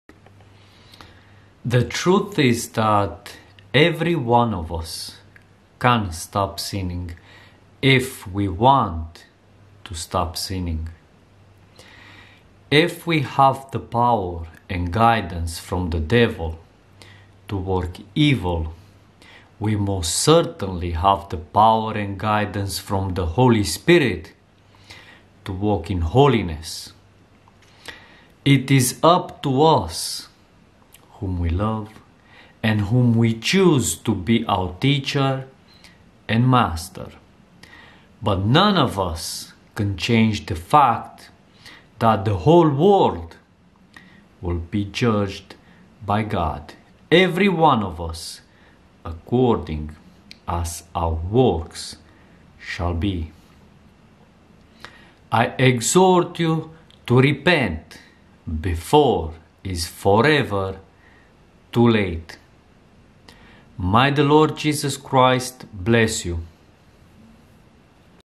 Dear Christian, YOU CANNOT AVOID THE TRUTH FOREVER!
SUBTITRAREA SE ACTIVEAZA DIN SETARI
LIMBA ROMANA MAI JOS
Dragă creștin, NU POȚI EVITA ADEVĂRUL PENTRU VECIE!
The truth is every one of us can stop sinning if we WANT to stop sinning. If we have the power and guidance from the devil to work evil we most certainly have the power and guidance from the Holy Spirit to walk in holiness. It is up to us whom we love and whom we choose to be our teacher and master but none of us can change the fact that the whole world will be judged by God, every one of us according as our works shall be.

I exhort you to repent before is forever too late!
May the Lord Jesus Christ bless you.

ROMANA

Adevărul este că fiecare dintre noi poate înceta să păcătuiască dacă VREM să încetăm să păcătuim. Dacă avem puterea și călăuzirea de la diavol pentru a face răul, cu siguranță avem puterea și călăuzirea de la Duhul Sfânt pentru a umbla în sfințenie. Depinde de noi pe cine iubim și pe cine alegem să fie învățătorul și stăpânul nostru, dar niciunul dintre noi nu poate schimba faptul că întreaga lume va fi judecată de Dumnezeu, fiecare dintre noi după cum vor fi faptele noastre.

Vă îndemn să vă pocăiți înainte să fie pentru totdeauna prea târziu!
Fie ca Domnul Isus Hristos să vă binecuvânteze.